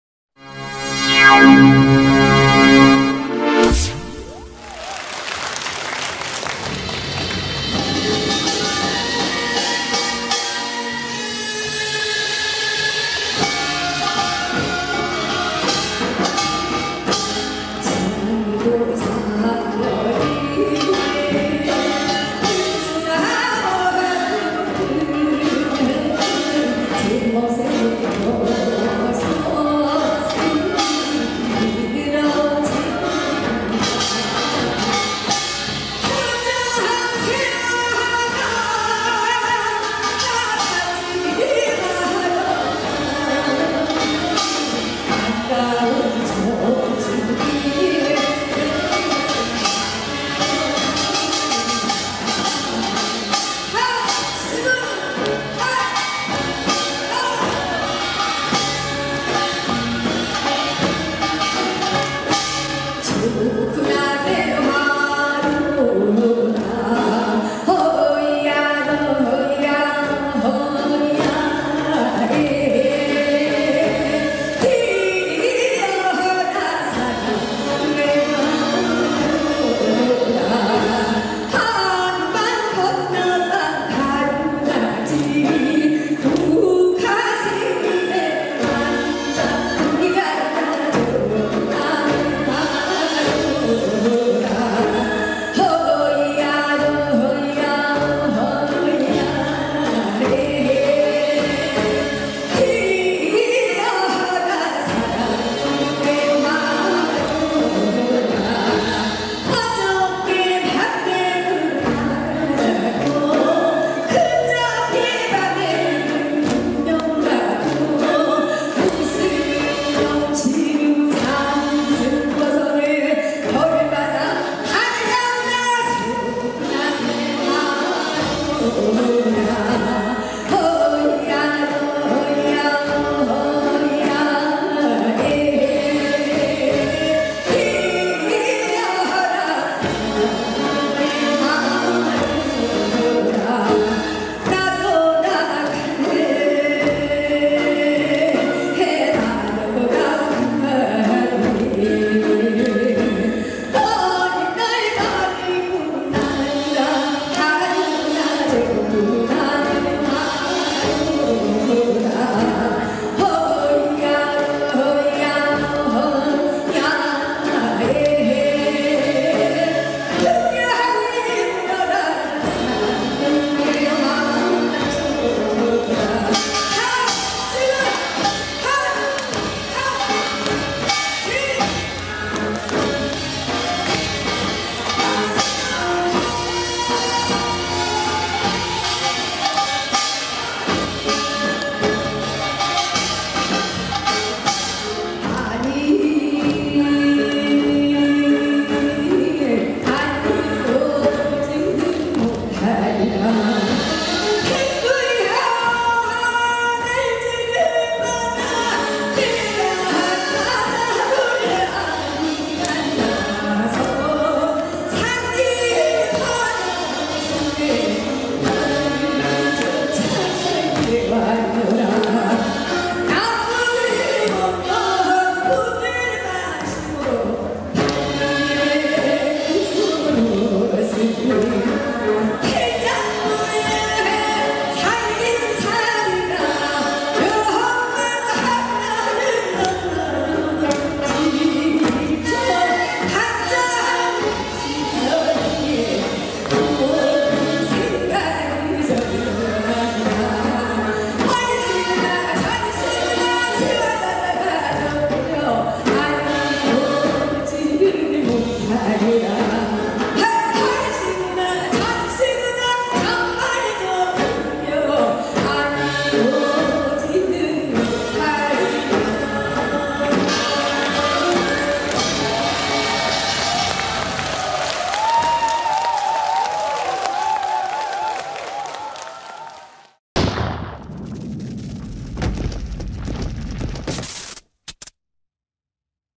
경기민요 명창 김영임의 민요메들리
2020 영동곶감축제 신년음악회에서, 4K, 고화질